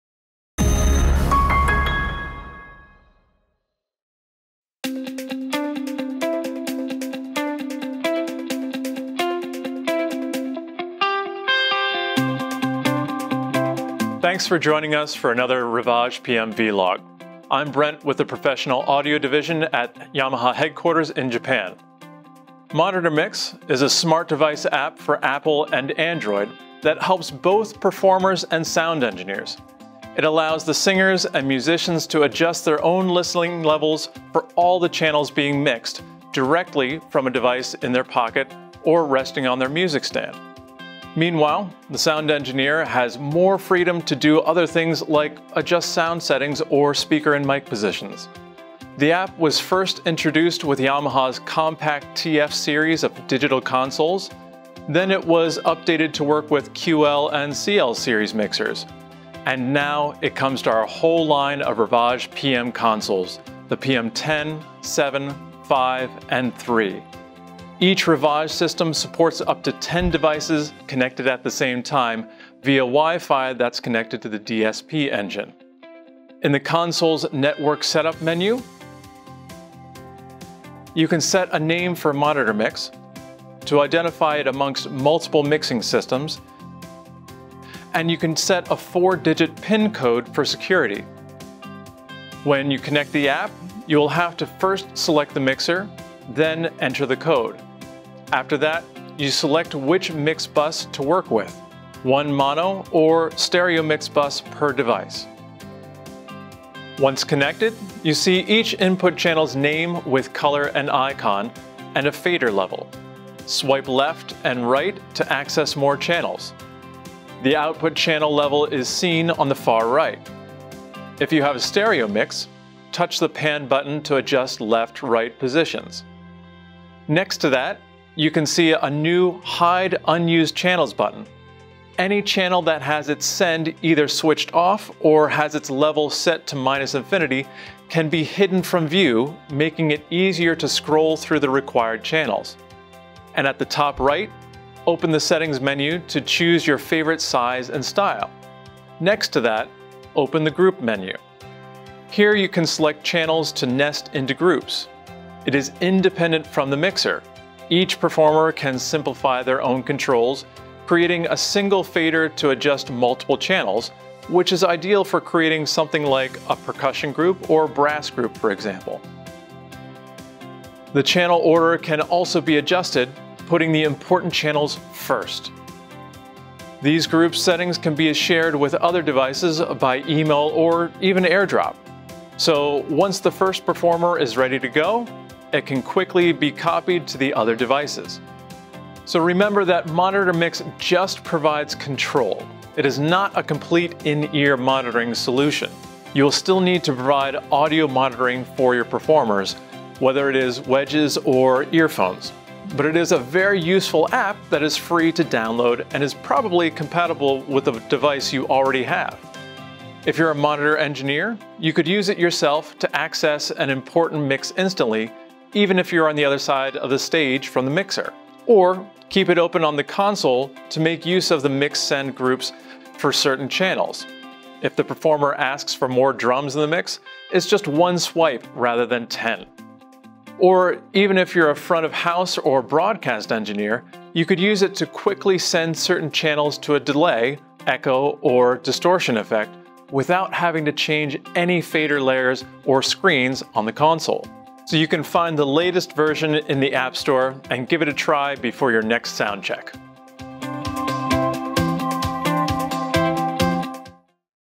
Yamaha RIVAGE PM Feature Vlog – MonitorMix
MonitorMix is a smart device app for Apple and Android, that helps both performers and sound engineers. It allows the singers and musicians to adjust their own listening levels for all the channels being mixed, directly from a device in their pocket or resting on their music stand. Check the video for more details!

The subtitles are available in the following languages:
English, 中文, Español, 日本語, Deutsch, Français, Русские, 한국어

You can find out more about RIVAGE PM series.